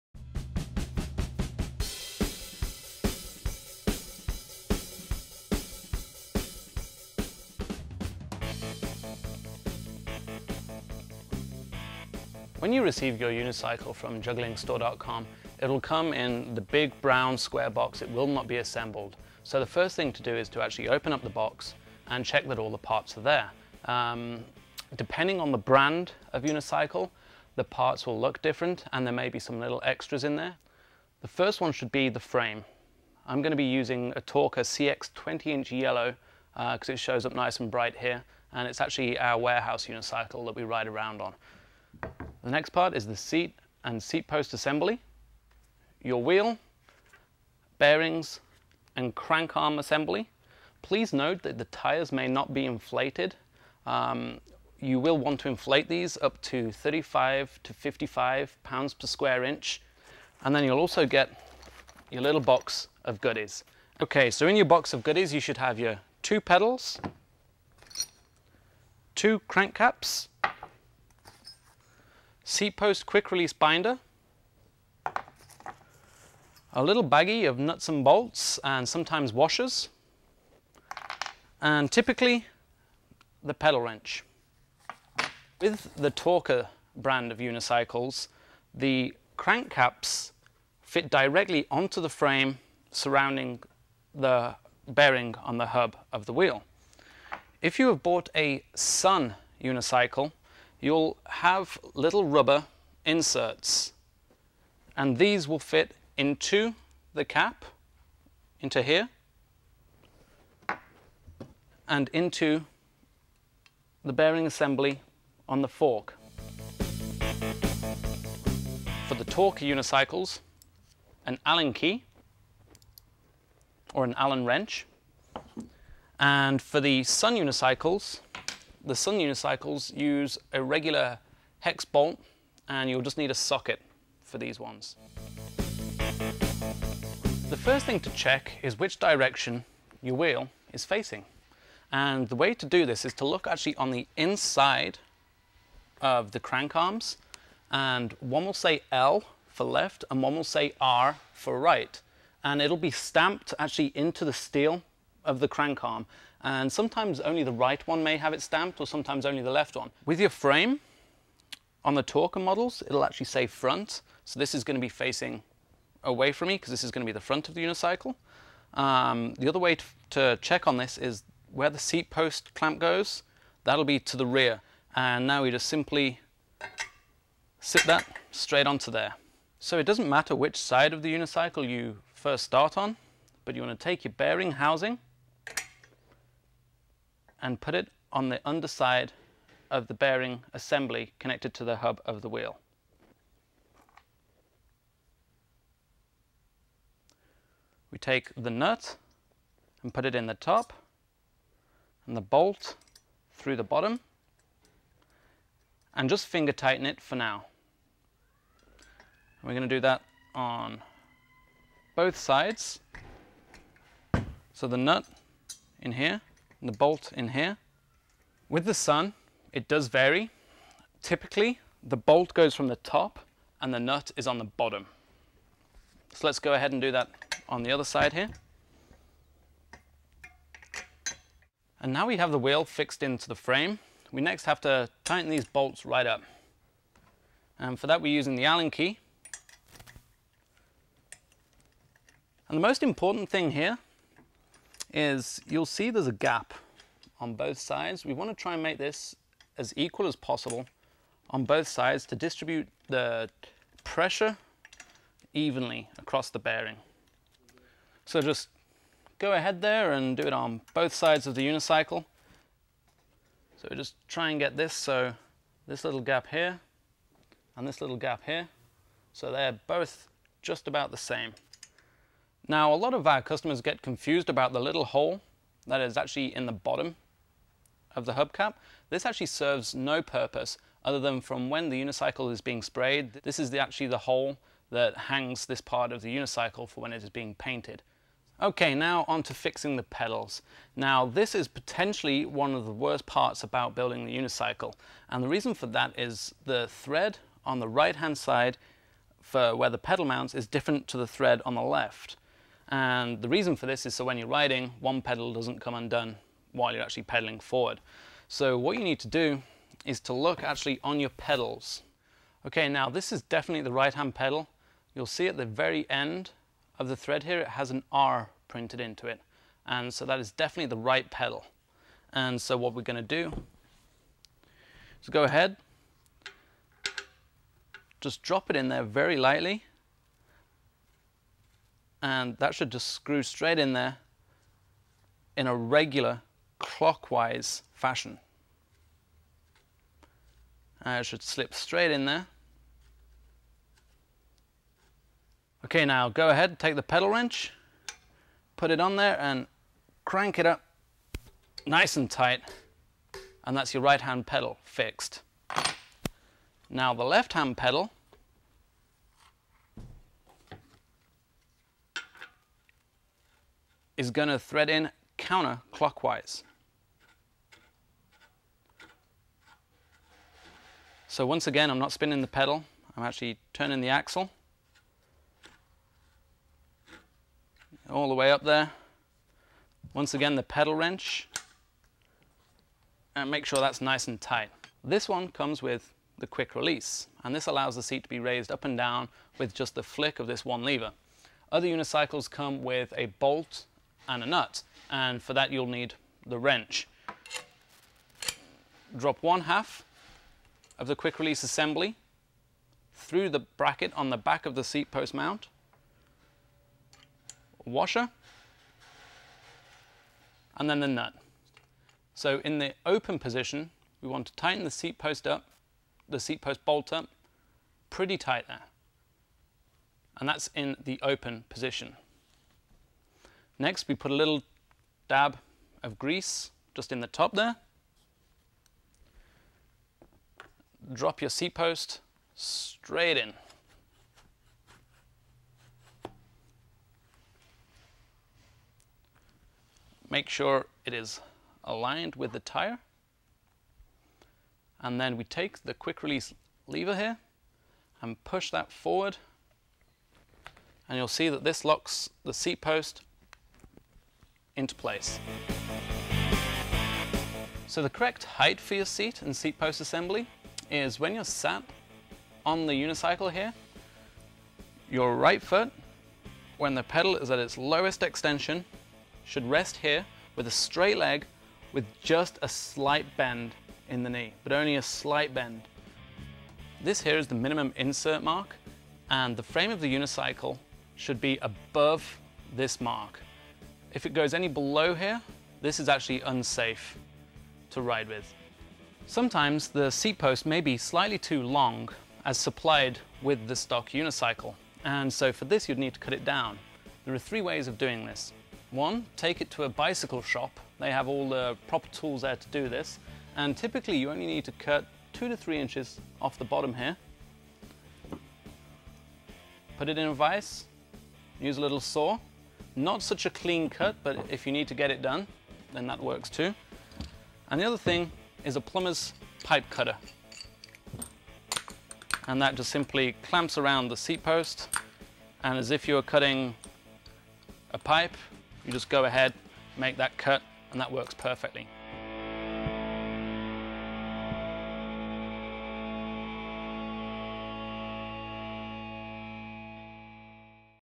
How to Build Your Unicycle
Need some help constructing your new uni? Check out this video starring our very own Aidan DeBradney Esquire II.